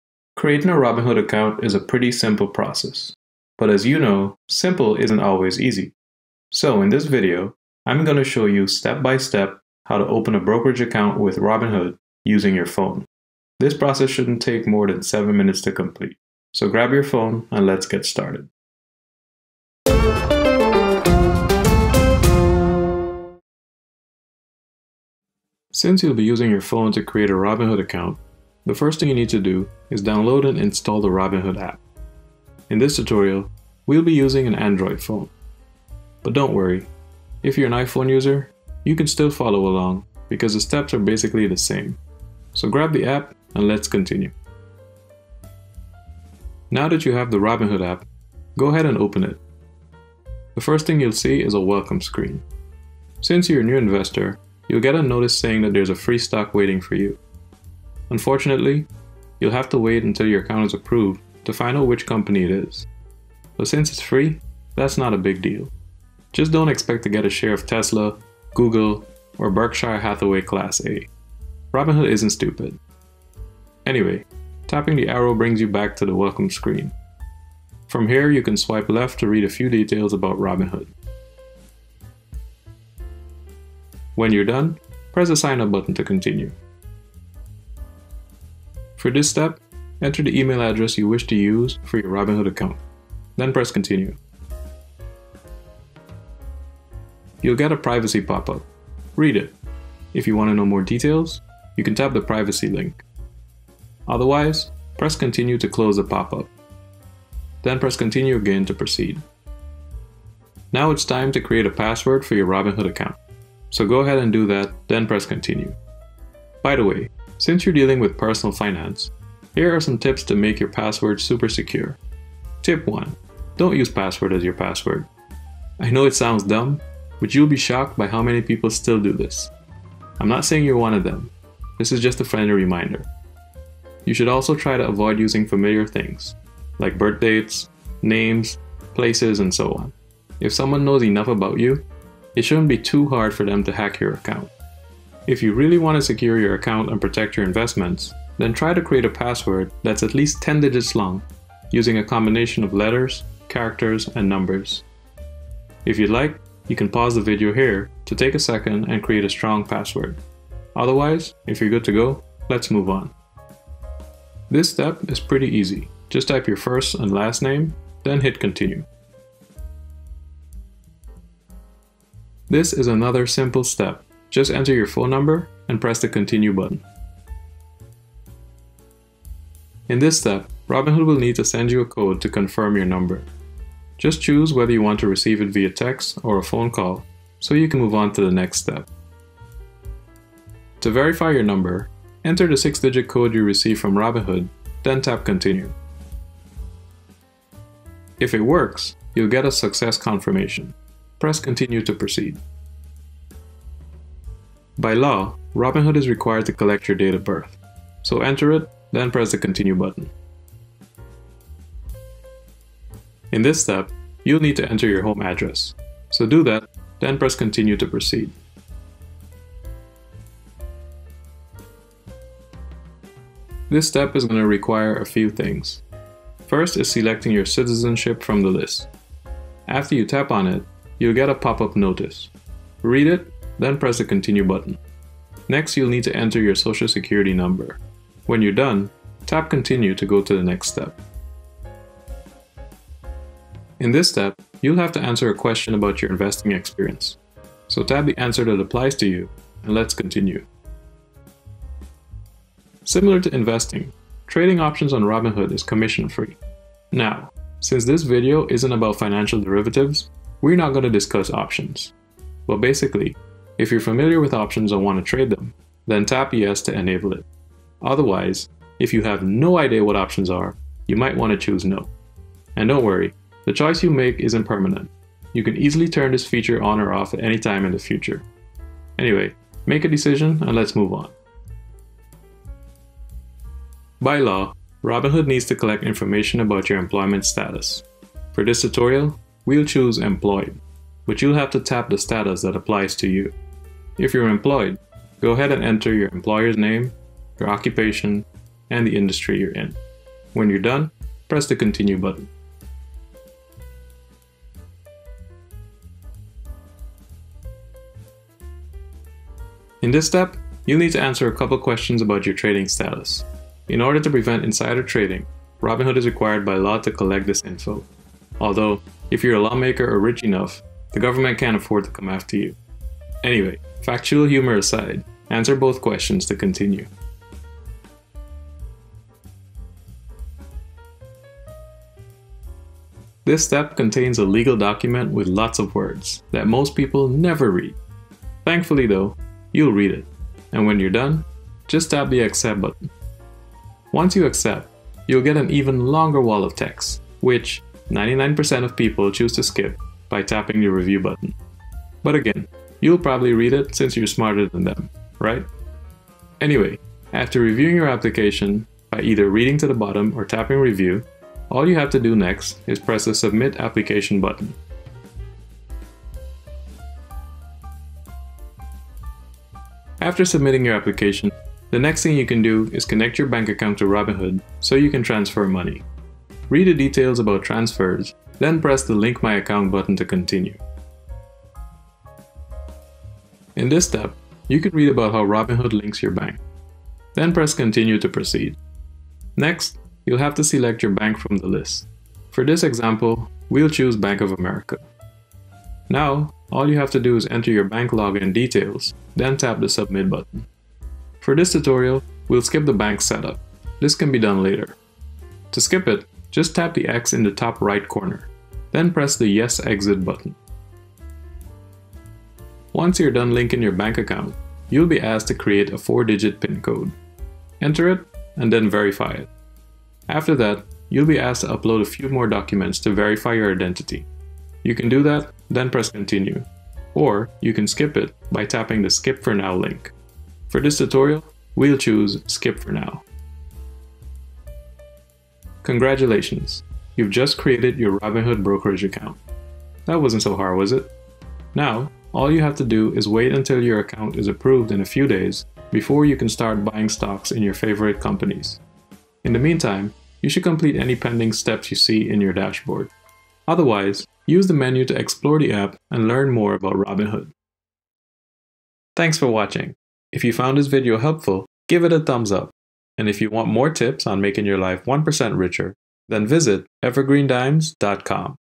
How to Open a Brokerage Account with Robinhood (Tutorial for Beginners)
This video will show you how to sign up for Robinhood using the Robinhood app. The platform's simplicity makes it one of the best brokerage accounts for beginners.

If you're a new investor looking to learn about the Robinhood sign up process, this video should help you. By watching this step-by-step tutorial, you'll be able to create a Robinhood brokerage account in minutes.

Once your Robinhood application is approved, you can start trading stocks with Robinhood and investing in companies.

⏱  Timestamps for Creating a Robinhood Account:

0:00 Intro
0:25 Download the app
0:46 Getting started with Robinhood
1:28 Enter your email
1:50 Create a password
2:43 Enter your full name
2:53 Add your phone number
3:03 Confirm your phone number
3:13 Verify your phone number
3:24 Enter your date of birth
3:34 Add your home address
3:44 Verify your citizenship & identity
4:03 Pick your level of investing experience
4:13 About trading options on Robinhood
4:44 Set your employment status
5:10 Verify your trading status
5:33 Review & submit your Robinhood application
6:09 Link your bank account to Robinhood
6:23 Add your bank details
6:52 Create a PIN code
7:15 Finish any tasks & explore the Robinhood app